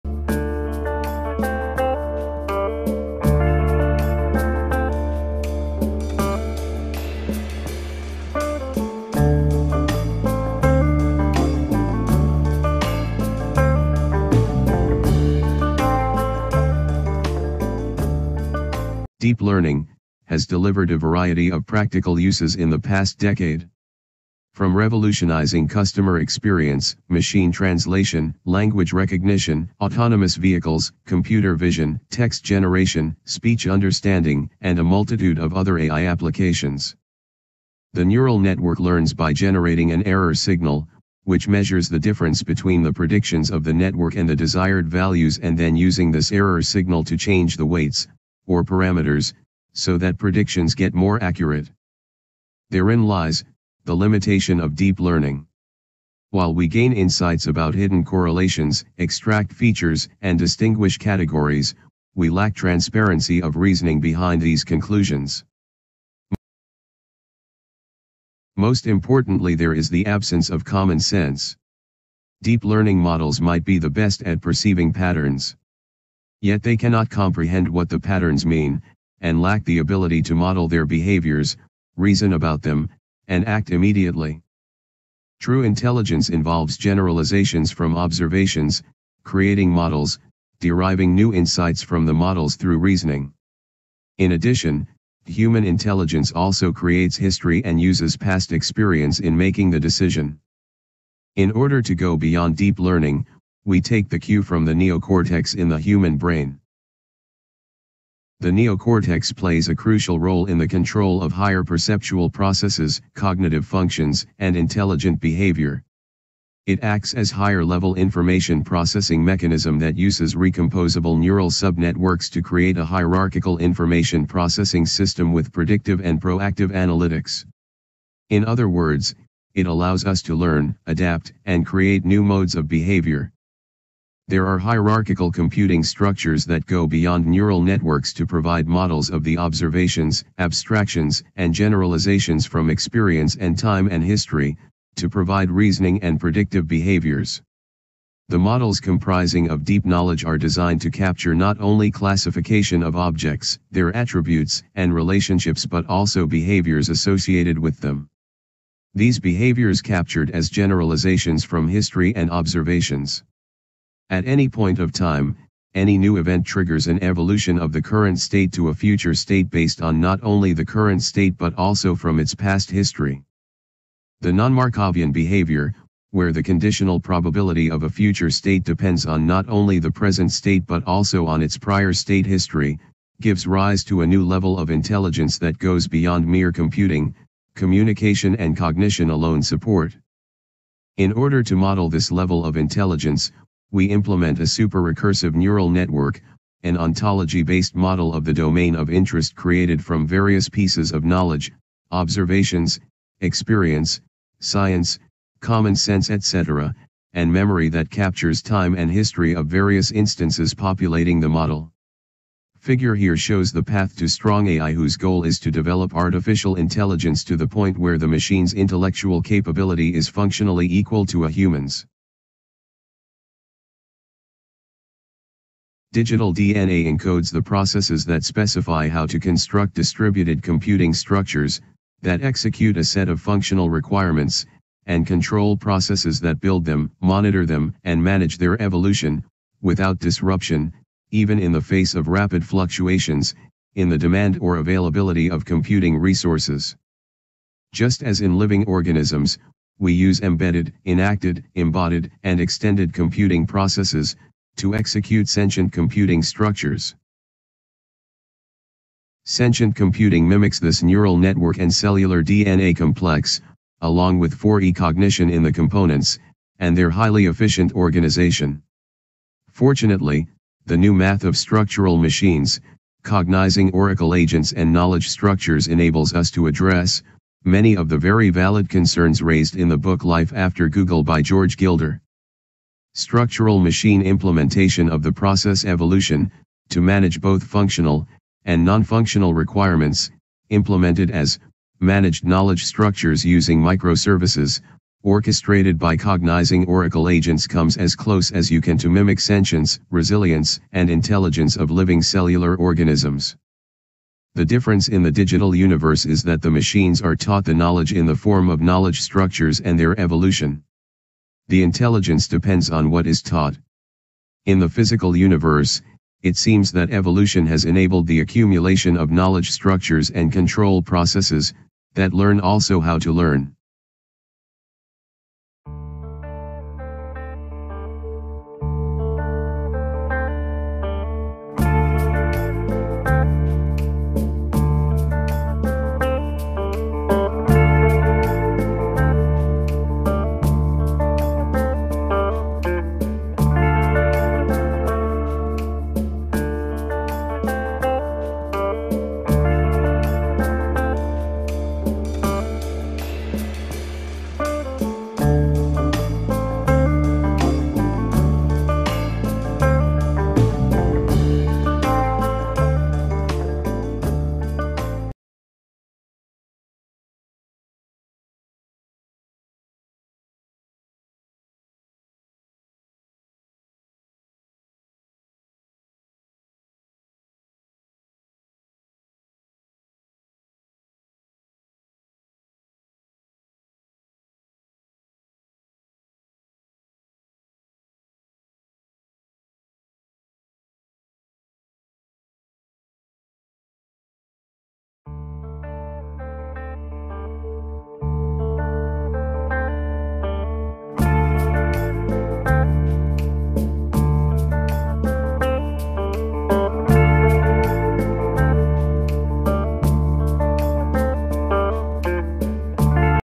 Deep Reasoning 0 fullhd Copy
Mikkilineni, R. Information Processing, Information Networking, Cognitive Apparatuses and Sentient Software Systems. Proceedings 2020, 47, 27.
This video is a companion to the paper detailing the thoughts on Sentient Computing

Deep learning, has delivered a variety of practical uses in the past decade. From revolutionizing customer experience, machine translation, language recognition, autonomous vehicles, computer vision, text generation, speech understanding, and a multitude of other AI applications.
The neural network learns by generating an error signal, which measures the difference between the predictions of the network and the desired values and then using this error signal to change the weights (or parameters) so that predictions get more accurate.
Therein lies, the limitation of Deep Learning. While we gain insights about hidden correlations, extract features and distinguish categories, we lack transparency of reasoning behind these conclusions. Most importantly there is the absence of common sense. Deep learning models might be the best at perceiving patterns. Yet they cannot comprehend what the patterns mean, and lack the ability to model their behaviors, reason about them, and act immediately.
True intelligence involves generalizations from observations, creating models, deriving new insights from the models through reasoning. In addition, human intelligence also creates history and uses past experience in making the decision.
In order to go beyond Deep Learning, we take the cue from the neocortex in the human brain. The neocortex plays a crucial role in the control of higher perceptual processes, cognitive functions, and intelligent behavior. It acts as higher-level information processing mechanism that uses re-composable neural subnetworks to create a hierarchical information processing system with predictive and proactive analytics. In other words, it allows us to learn, adapt and create new modes of behavior.
There are hierarchical computing structures that go beyond neural networks to provide models of the observations, abstractions and generalizations from experience and time and history, to provide reasoning and predictive behaviors. The models comprising of deep knowledge are designed to capture not only classification of objects, their attributes and relationships but also behaviors associated with them. These behaviors captured as generalizations from history and observations. At any point of time, any new event triggers an evolution of the current state to a future state based on not only the current state but also from its past history.
The non-Markovian behavior, where the conditional probability of a future state depends on not only the present state but also on its prior state history, gives rise to a new level of intelligence that goes beyond mere computing, communication and cognition alone support.
 In order to model this level of intelligence, we implement a superrecursive neural network, an ontology-based model of the domain of interest created from various pieces of knowledge (observations, experience, science, common sense etc.) and memory that captures time and history of various instances populating the model.  Figure here shows the path to strong AI whose goal is to develop artificial intelligence to the point where the machine's intellectual capability is functionally equal to a human's.